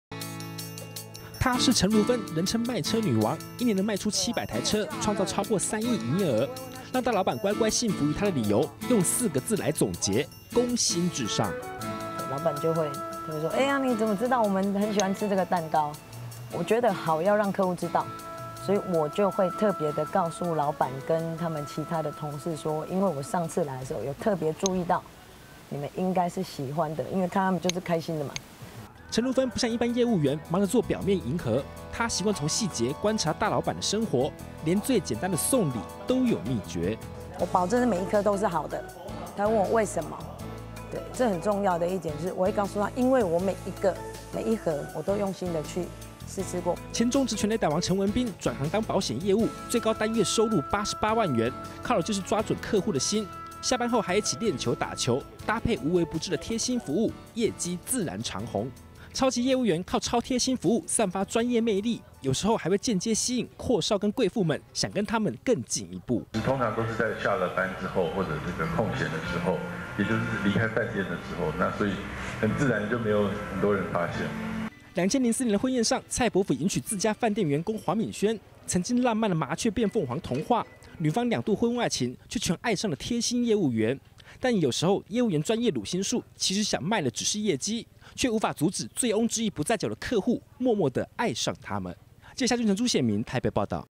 20180627中天新聞 櫃姐、業務「攻心」衝業績 闊少、貴婦常「愛上他/她」
為何闊少或是貴婦，都會巧合愛上業務員？，其實「超級業務」最強的地方，就在無微不至的「攻心術」，這種讓人很難抵抗的「專業魅力」，明明只是想促銷業績，卻有客戶會主動陷入情網，像賴英里媳婦跟男業務員曖昧，就是個最佳例子。